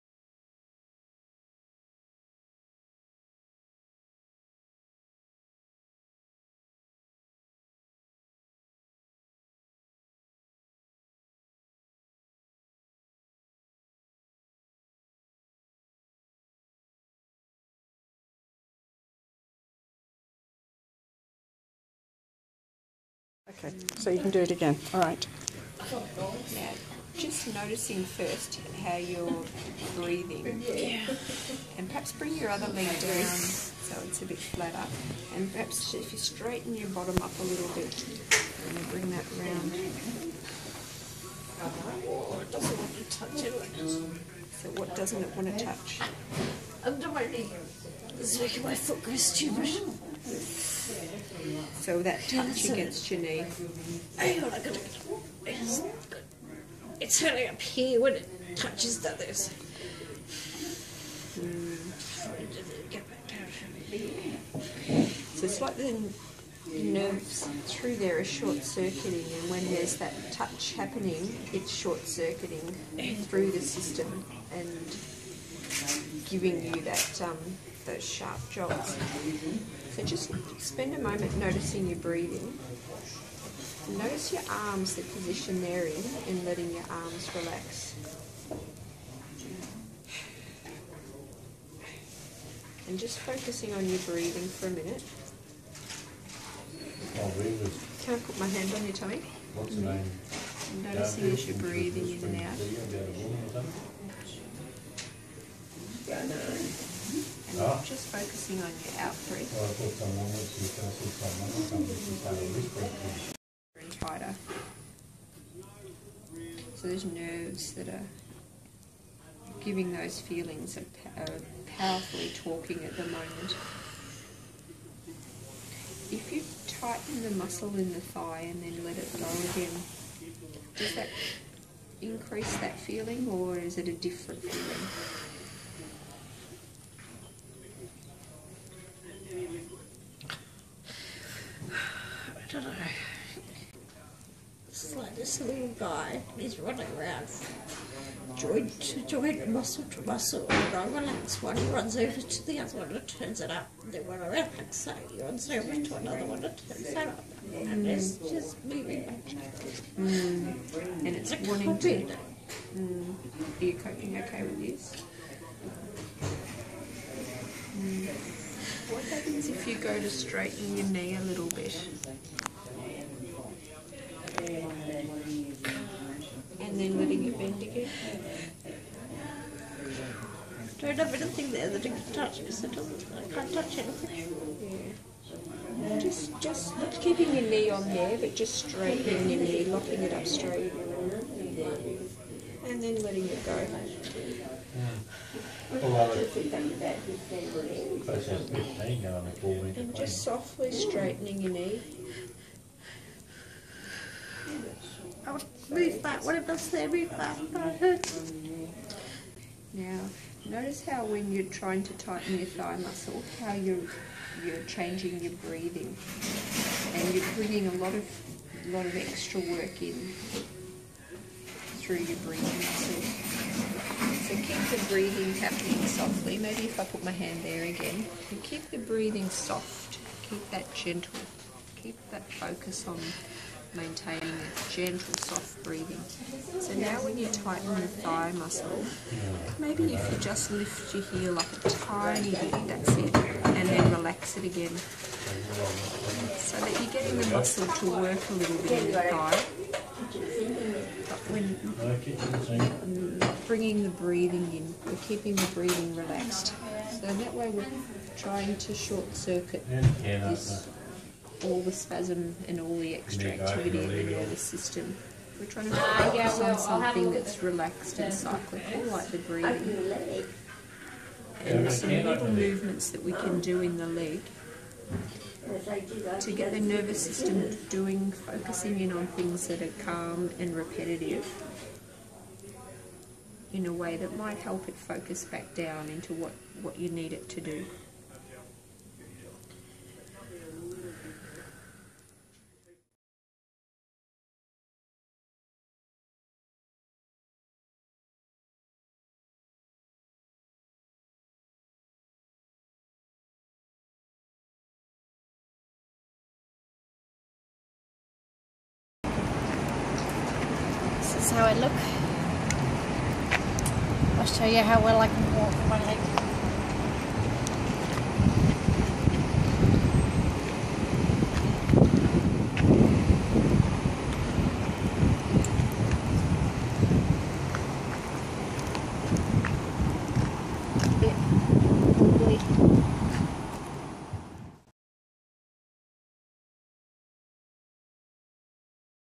RSD/CRPS Pain Attack in Hospital During Ketamine Infusion - Shared to Spread Awareness
An example of the RSD pain flare. This one happened while I was in hospital for a ketamine infusion, 15 years after my CRPS began.

It's hard to watch but its important to share, to spread awareness of this horrible condition so a cure can be found.

Uploaded 15 years since my RSD/CRPS began.